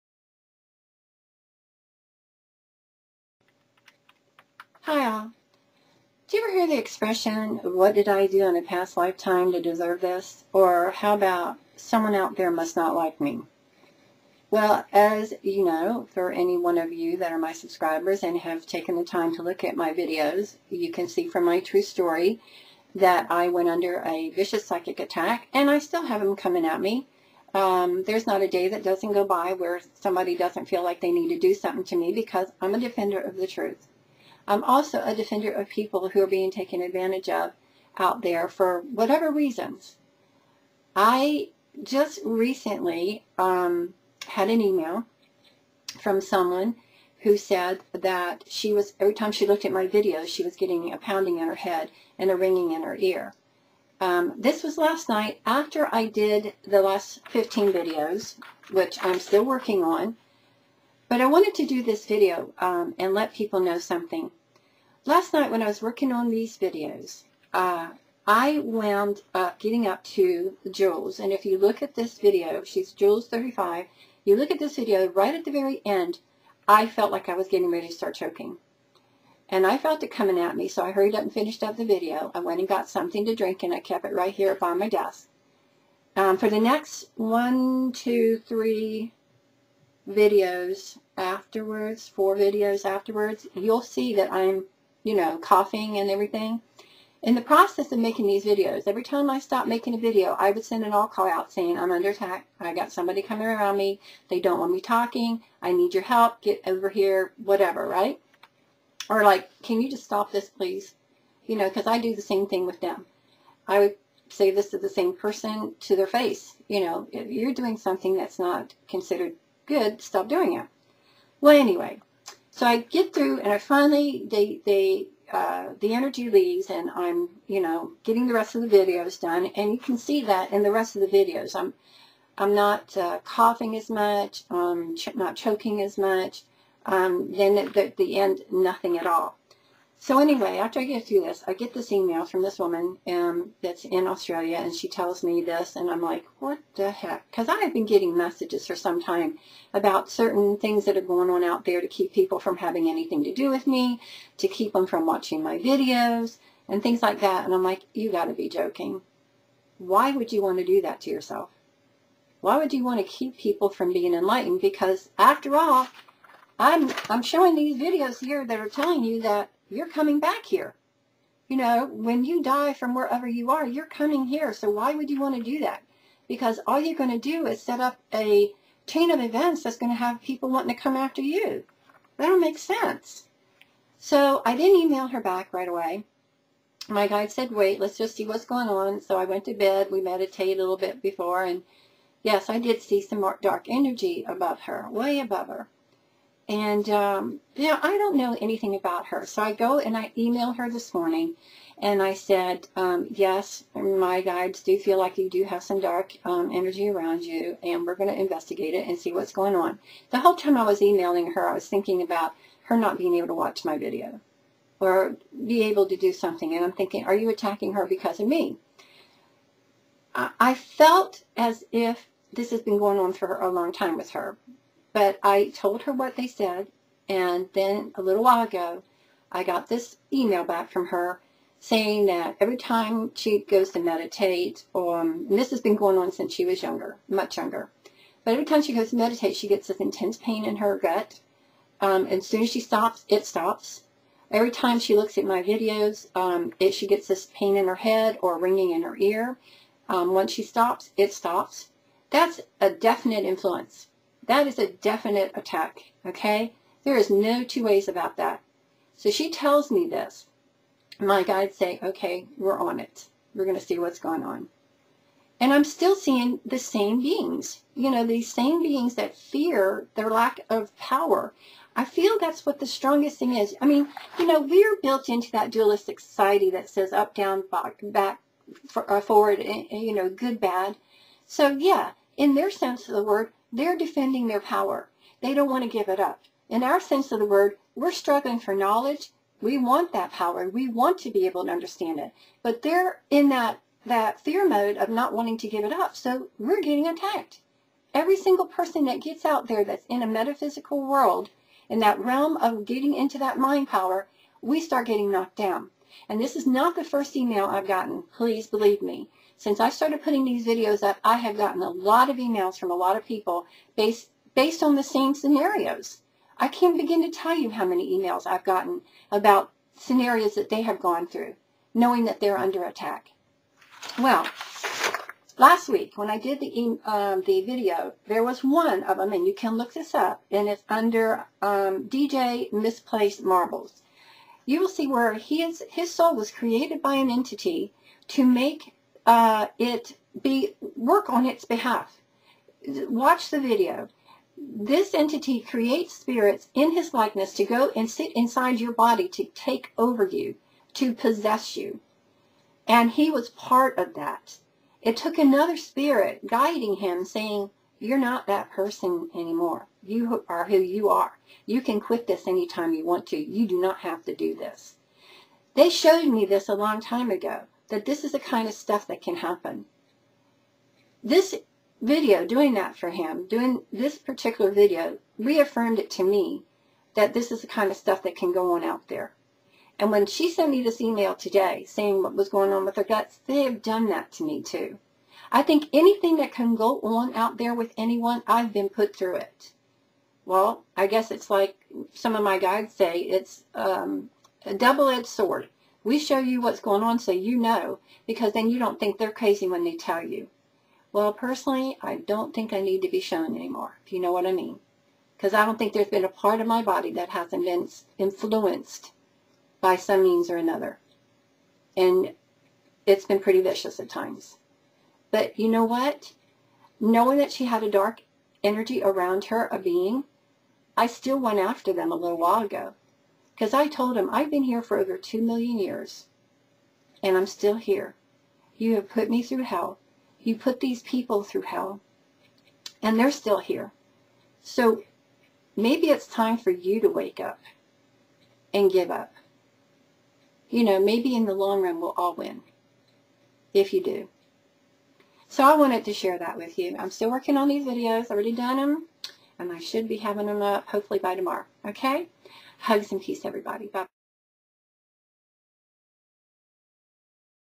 Urgent Message - Please Watch
PLEASE READ... Again today, I get an email from one of my subscribers that has and is experiencing attacks to her body - the message is coming through that now it's because she is watching my videos - or at least trying to. I went under one while doing these last videos, as well.

Over the past year I have received similiar emails and have done all I can do to help and protect people and the biggest thing that I hear off the universe is the fear that these entities have of losing their abilities of lording it over us. Well, based on my soul start videos - us is also them. They need to start realizing that they are attacking us - but it's someone they may know from the past - maybe someone dear to them too.